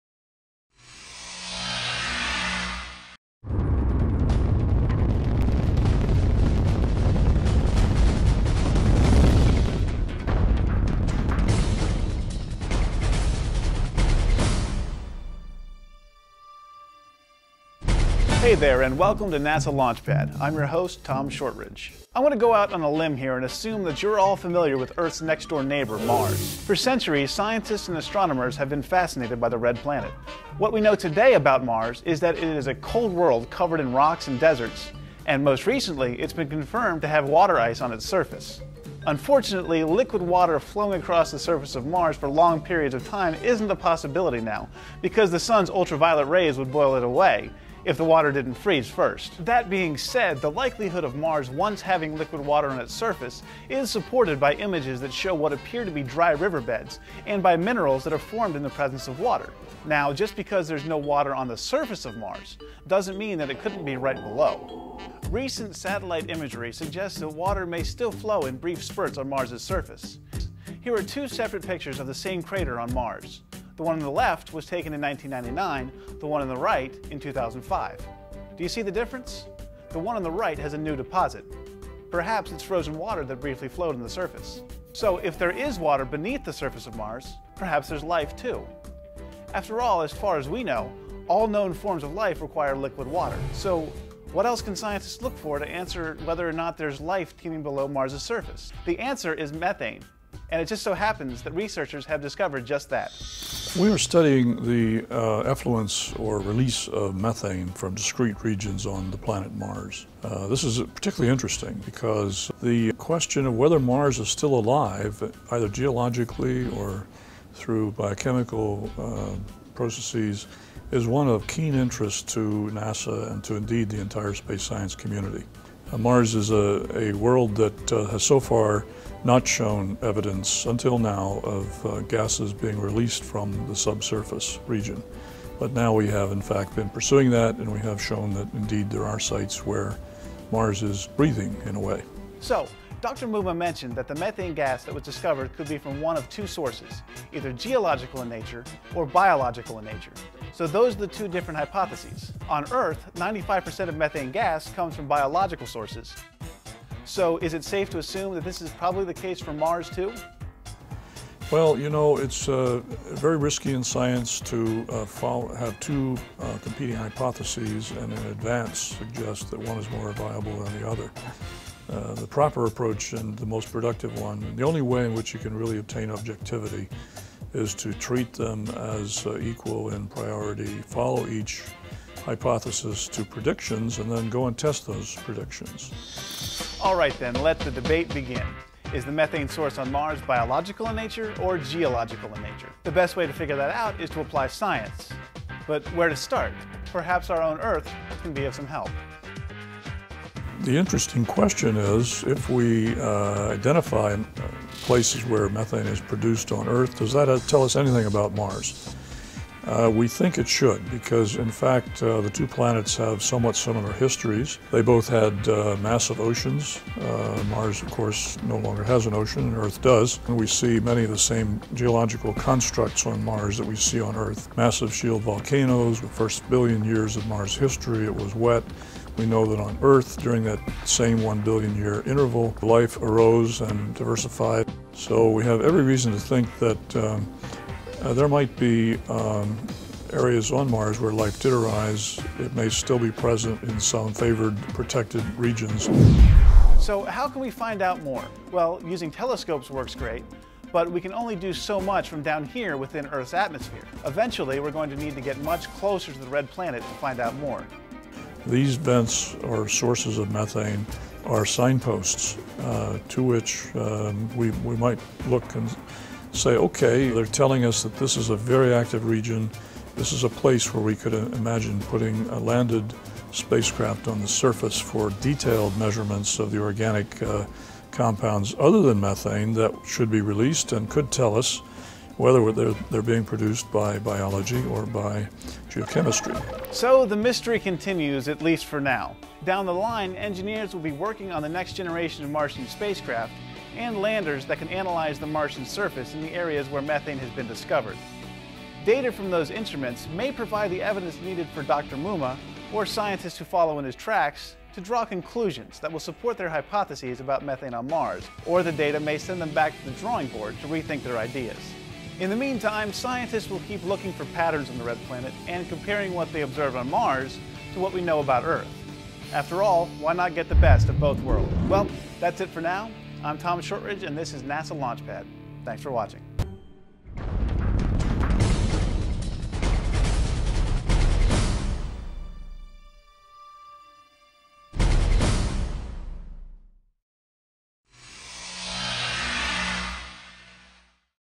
Launchpad: Methane on Mars
Is Mars a living planet, either geologically or biologically? See how NASA scientists are investigating the recent discovery of water ice and methane plumes on Mars to test their hyppotheses about the similarities between Earth and Mars.